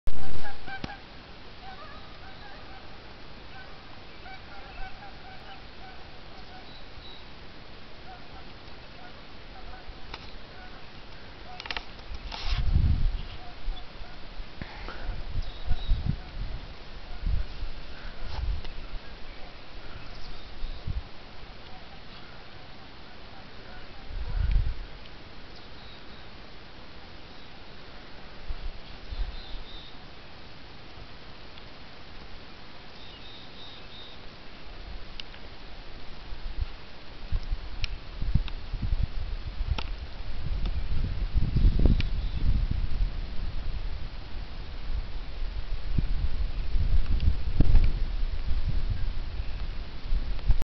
slough meadow on the deschutes river
where i caught my 21 inch brown trout today!